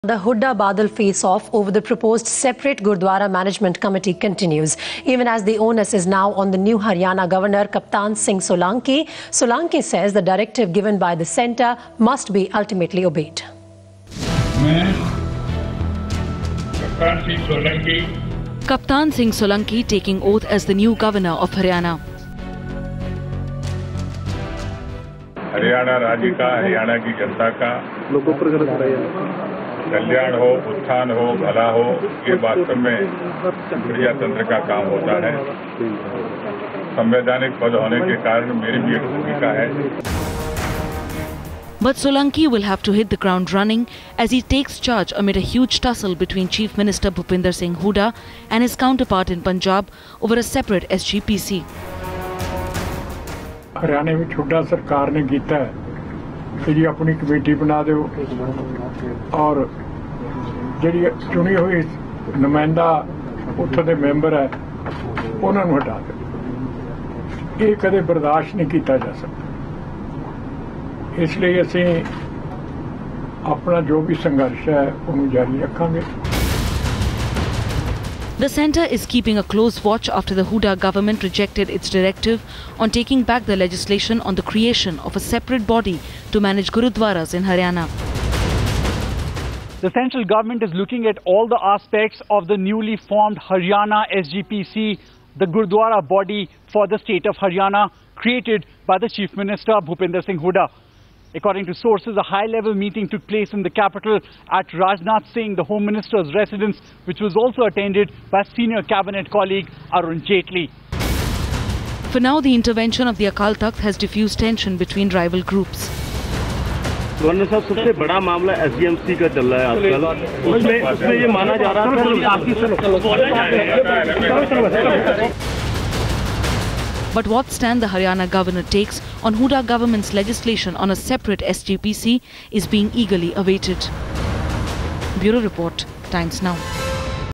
Tussle over Sikh Gurdwara body
The Hooda-Badal face off over the proposed separate Gurdwara Management Committe continues. Even as the Onus is now on the New Haryana Governor, Kaptan Singh Solanki, Solanki says the directive given by the Centre must be ultimately obeyed. Social Media Links :-